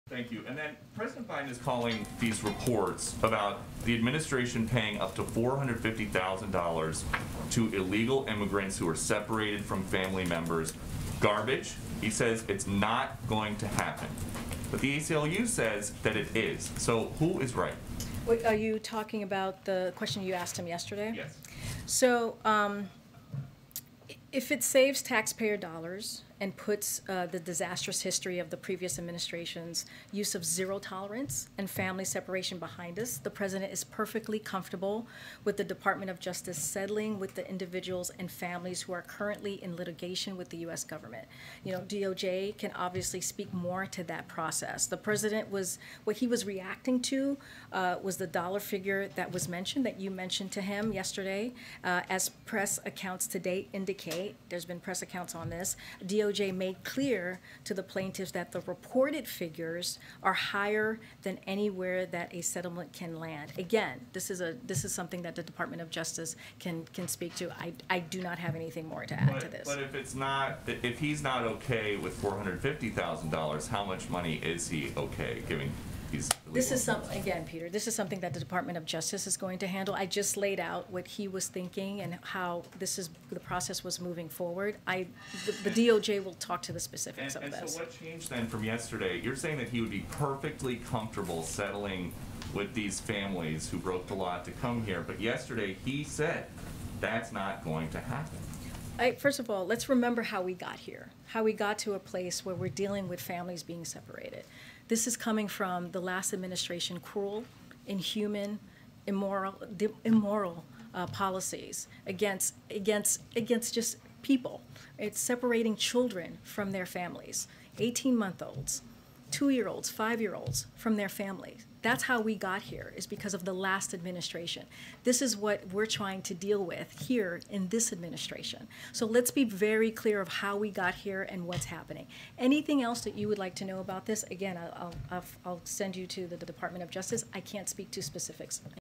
Deputy Press Secretary Karine Jean-Pierre goes over Peter Doocy's "garbage" report
Deputy Press Secretary Karine Jean-Pierre goes over Peter Doocy's "garbage" report he asked President Biden about yesterday.